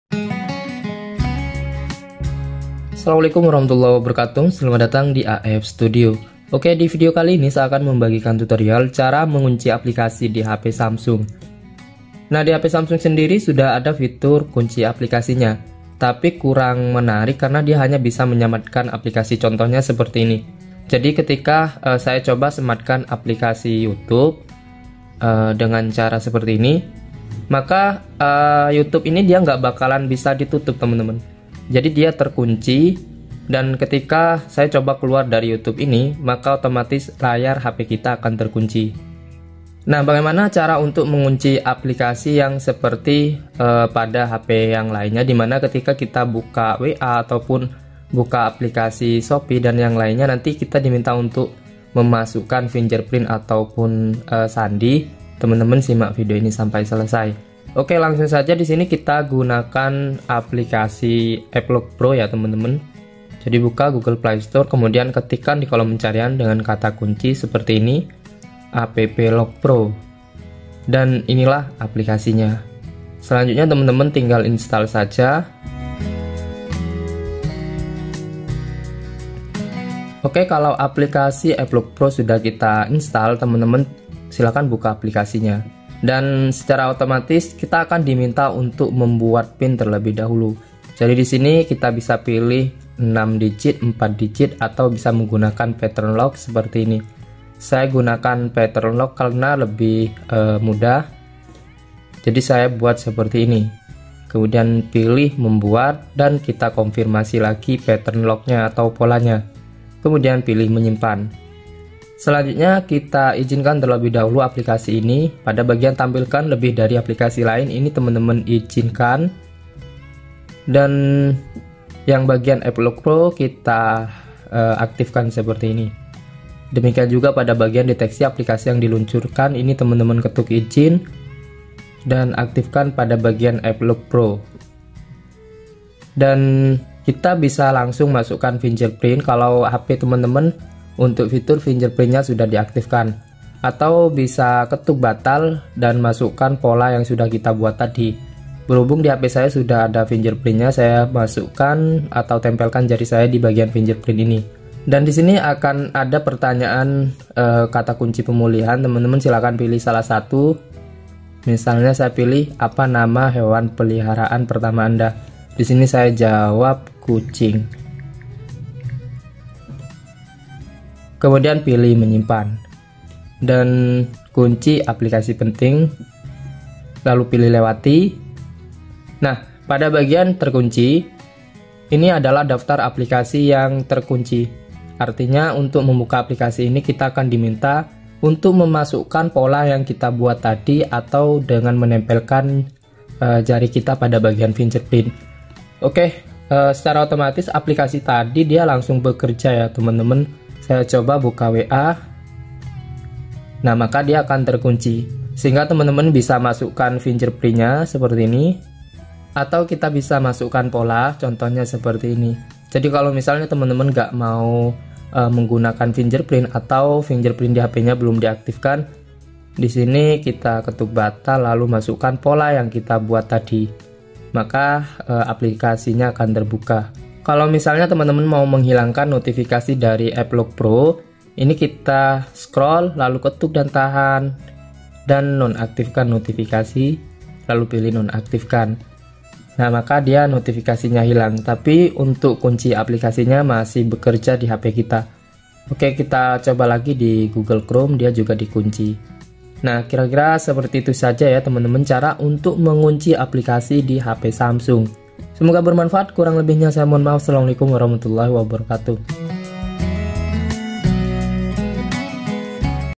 Cara Mengunci Aplikasi Di Hp Samsung
Berikut saya bagikan tutorial cara mengunci aplikasi di hp samsung. Temen-temen bisa kunci aplikasi WA, youtube, galeri dan berbagai aplikasi lainnya dengan sidik jari atau sand-.